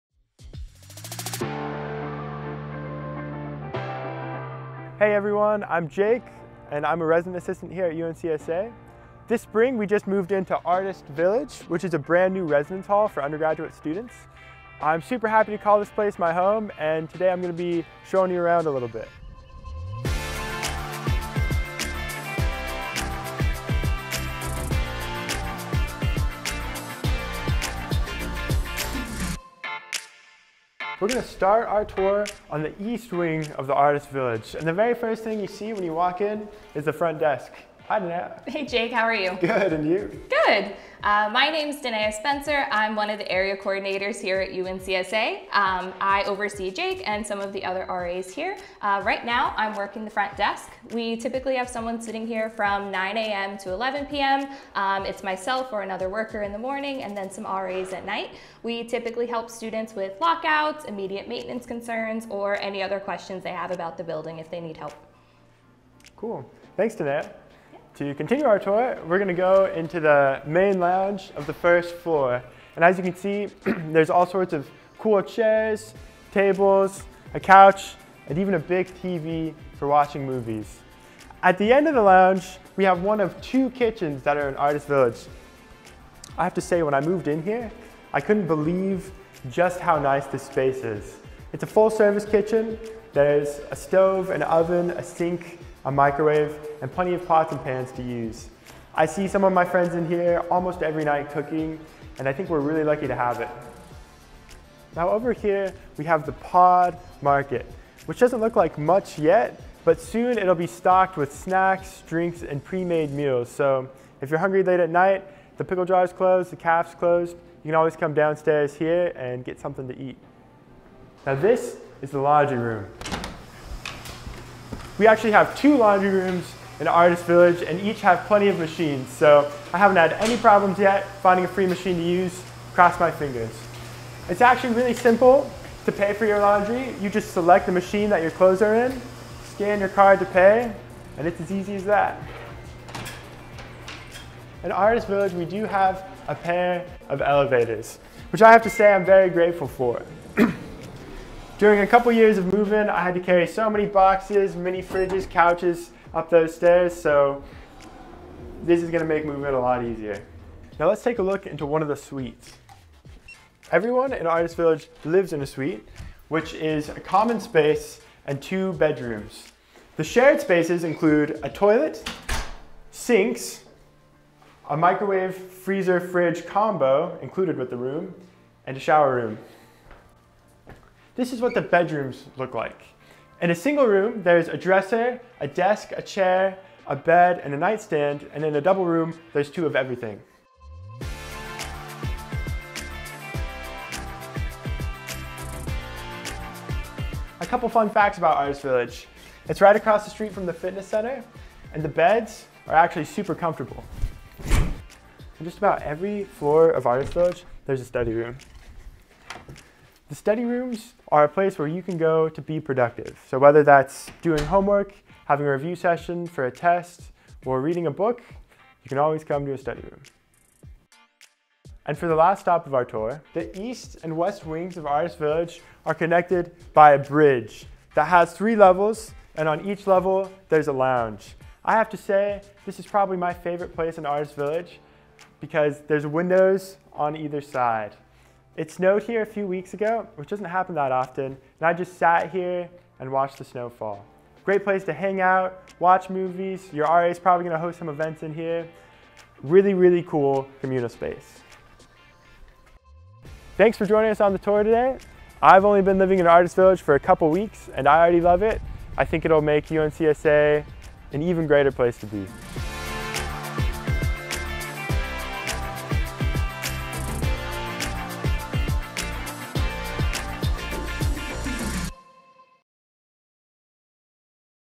Inside Artist Village: A tour of our new residence hall
Join Resident Assistant and School of Music student Jake Anderson for a tour of the recently-opened Artist Village residence hall at UNCSA. In Artist Village, undergraduate students live in suite-style across two wings and five floors.